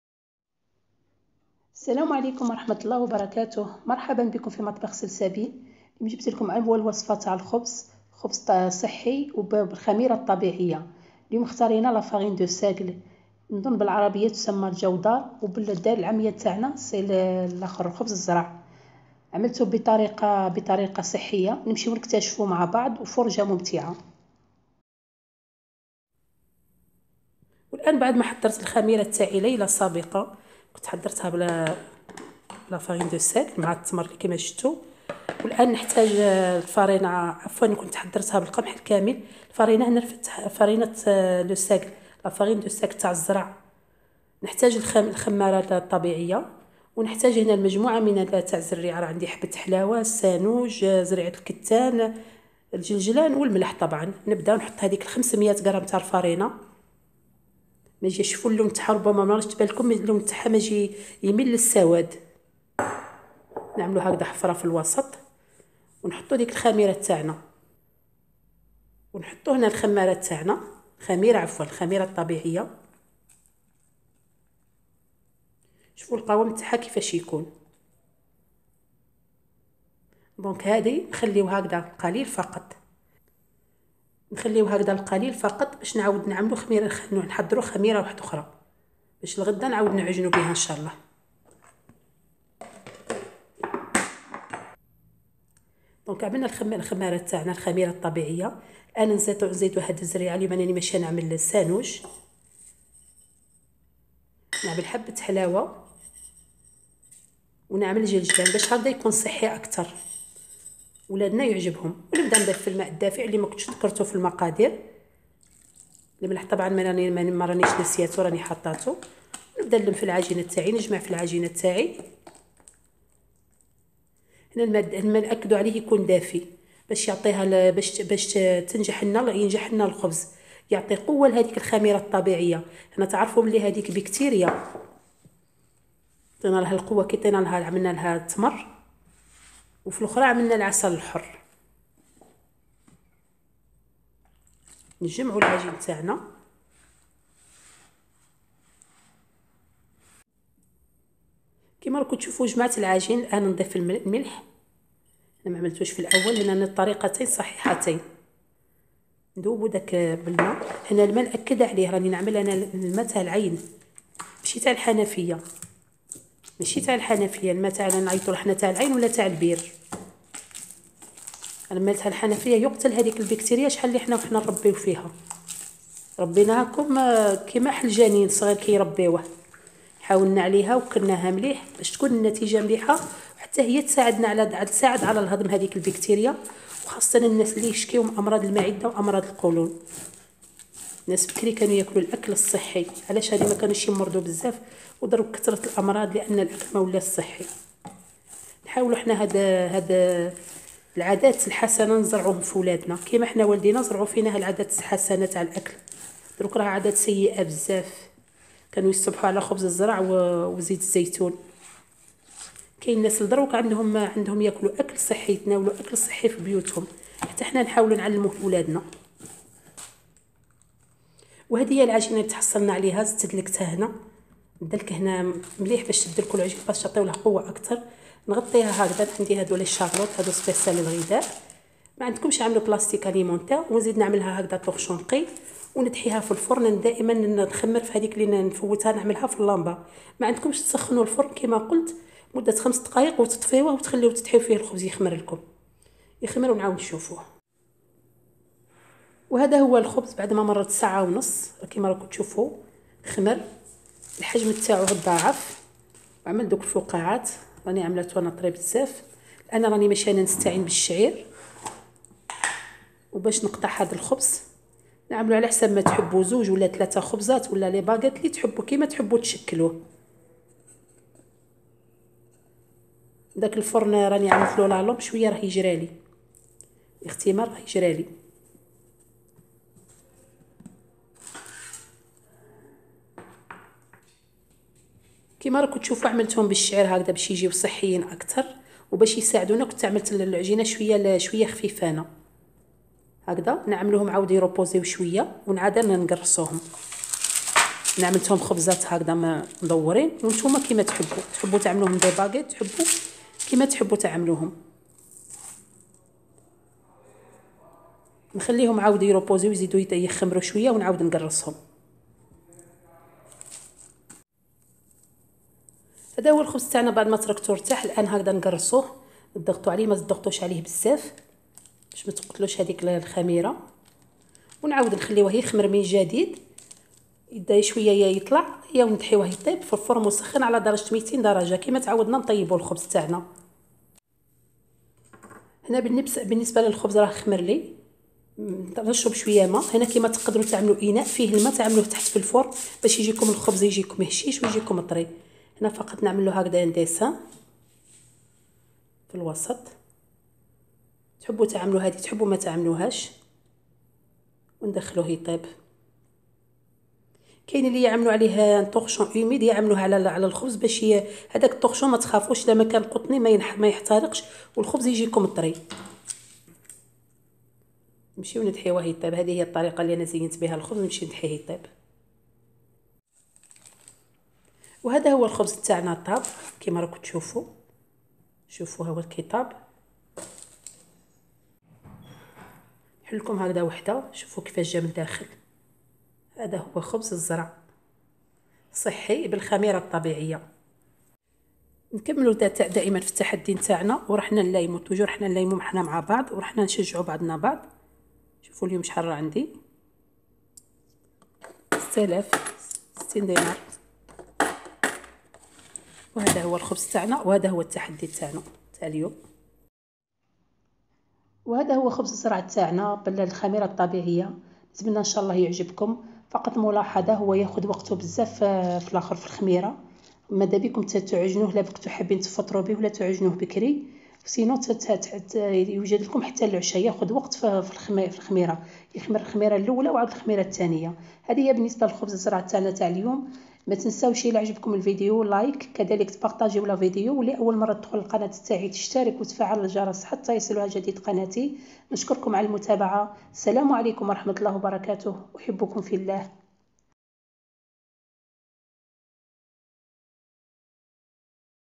خبز الزرع ( الجودار ) الصحي 100%👌👌👌بالخميرة الطبيعية والحبوب المختلفة وبمذاق رائع 🤗🤗
Ingrédients  :
500 g de farine de saigle
Levure naturel
Sel
L'eau  tiède
Grain de sésame
Grain de  nigelle
Gain  d'anis
L"orge
المقادير  :
500غ دقيق الجودار ( الزرع )
خميرة طبيعية
ملح
ماء دافئ
الجلجلان
سنوج
حبة حلاوة
الشعير